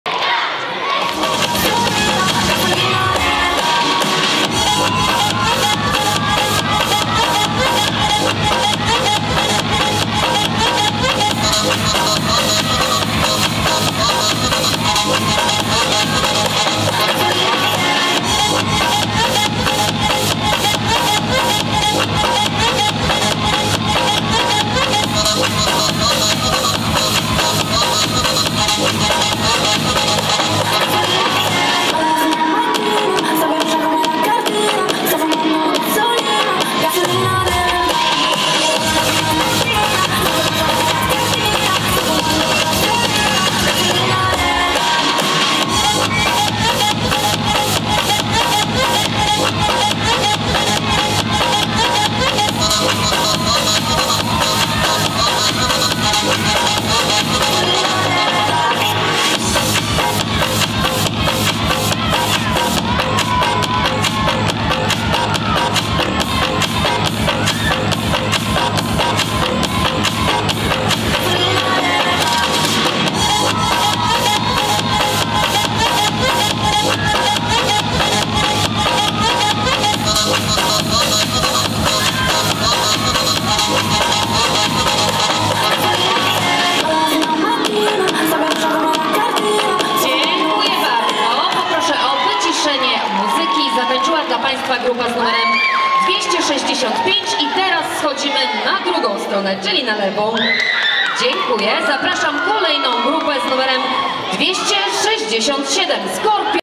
Dance Game 2023 Mini formacje Dzieci Mini Princess - Spark Team Kids 2M
Dance Game 2023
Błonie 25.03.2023

Disco Dance Mini formacje Dzieci
Mini Princess - Spark Team Kids   2 miejsce
Egurrola Dance Studio Warszawa